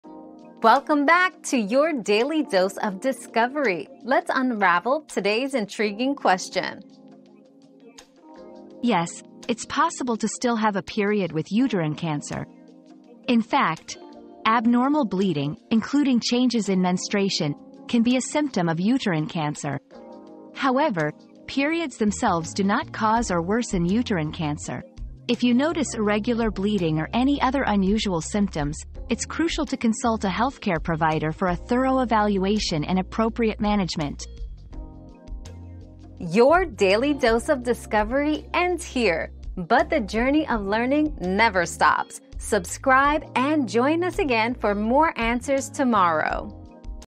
Can you still have a period with uterine cancer?
Can You Still Have a Period with Uterine Cancer? 👉 Periods and Uterine Cancer 👉 Discover if it's possible to still have a period while dealing with uterine cancer, and learn about the importance of seeking medical advice for any irregular bleeding or unusual symptoms.

Laura S. Harris (2024, February 2.) Can you still have a period with uterine cancer?
    🌐 AskAbout.video